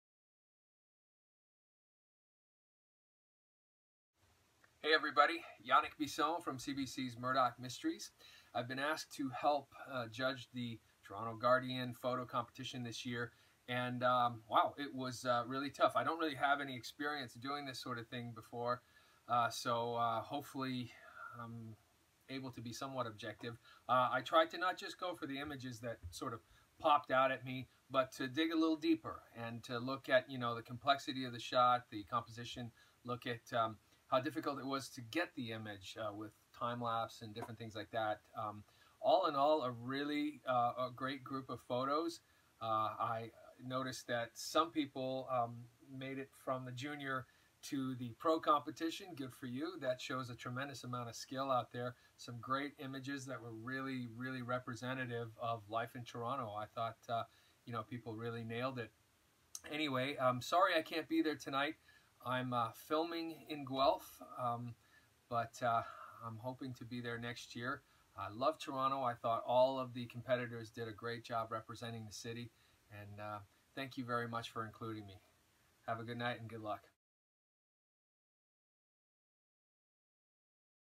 Toronto Guardian Photo Competition Judge Yannick Bisson of CBC's Murdoch Mysteries
Yannick Bisson sent us this message for competitors of the Toronto Guardian Photo Competition.